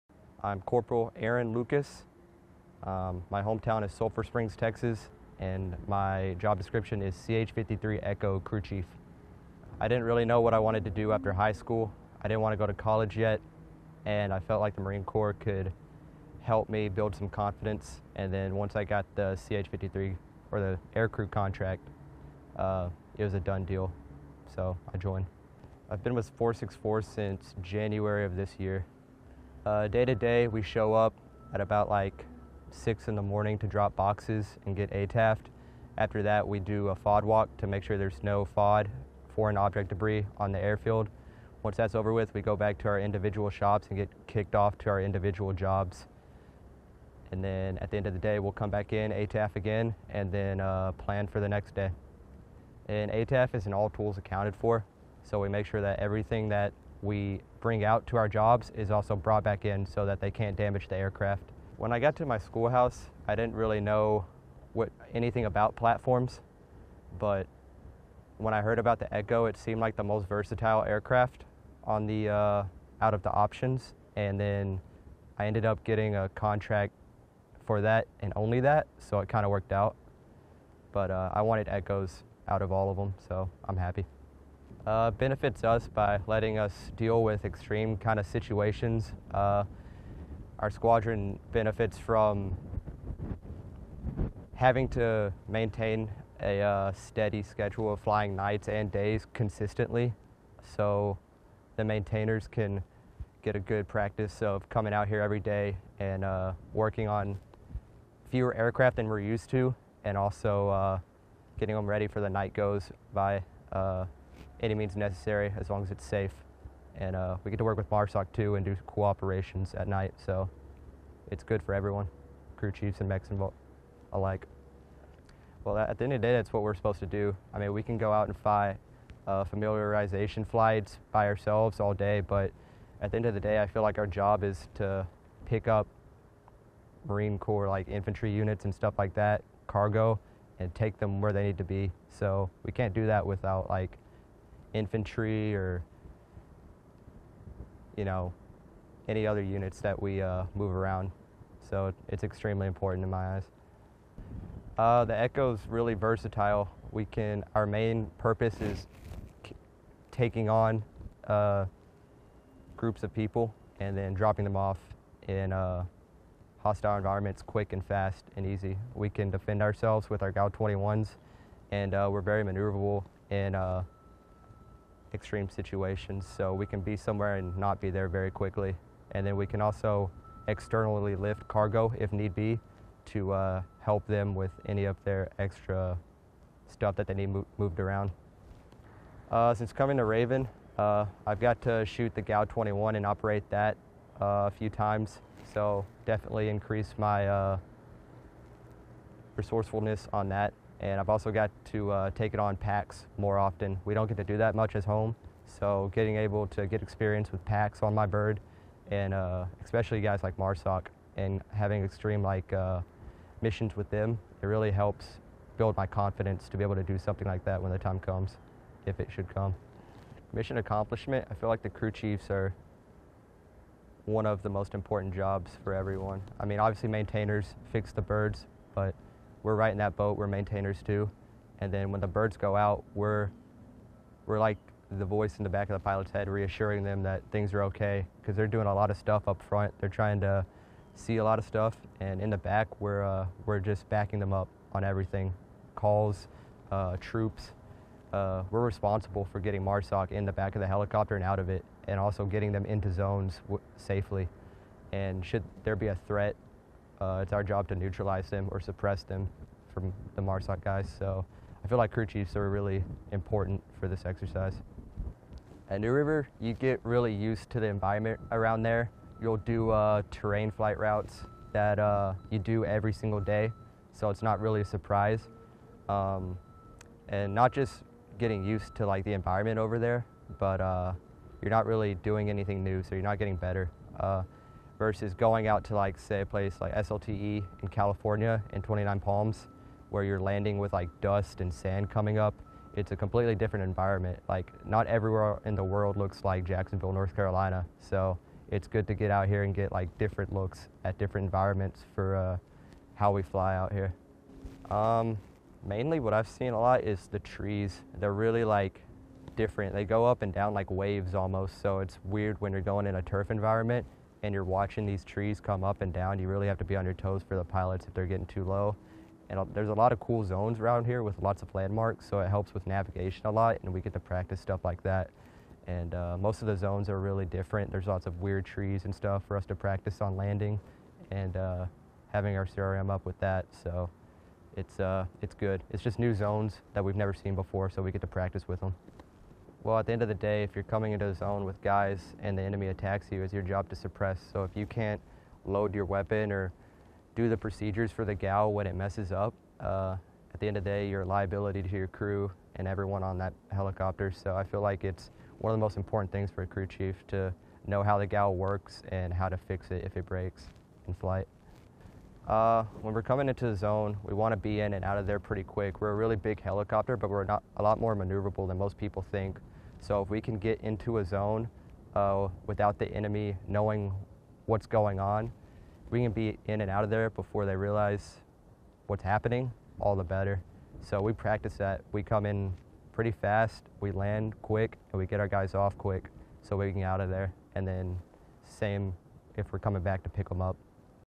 Interview with Cpl. Aaron Lucas, a CH-53E Super Stallion crew chief with HMH-464
U.S. Marine Corps Cpl. Aaron Lucas, a CH-53E Super Stallion crew chief with Marine Heavy Helicopter Squadron (HMH) 464, discusses the role of a crew chief and the experience he has gained during Exercise Raven 23-1 at an undisclosed location, Oct. 13, 2022.

(UNDISCLOSED LOCATION)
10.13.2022

HMH-464 provided assault support for Marine Forces Special Operations Command during Exercise Raven 23-1 to enhance combat readiness in an unfamiliar expeditionary environment. HMH-464 is a subordinate unit of 2nd Marine Aircraft Wing, the aviation combat element of II Marine Expeditionary Force. (U.S. Marine Corps video by Pfc. Rowdy Vanskike)

Film Credits: Video by Pfc. Rowdy Vanskike
2nd Marine Aircraft Wing